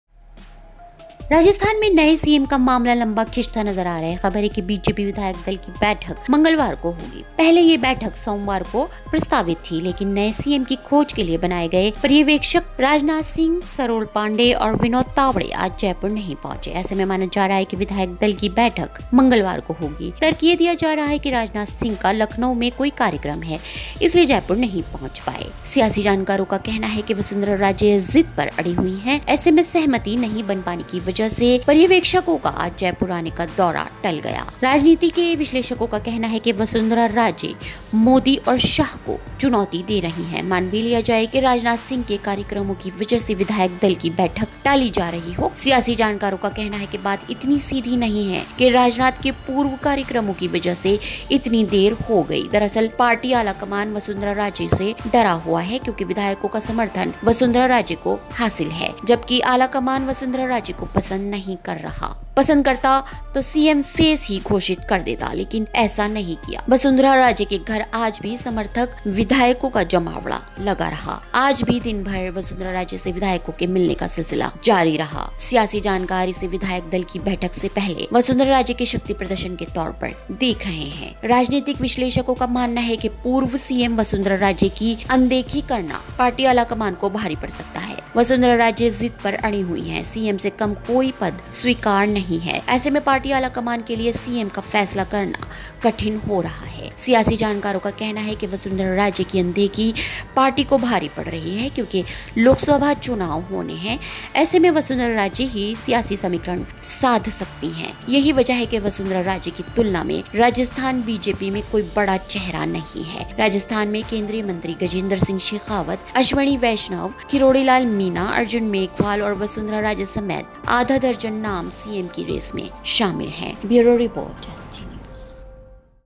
Rajasthan New CM Name: Vasundhara Raje की Modi और Amit Shah को चुनौती | CM पद से नीचे कुछ मंजूर नहीं
Rajasthan New CM Name: Vasundhara Raje की Modi और Amit Shah को चुनौती | CM पद से नीचे कुछ भी मंजूर नहीं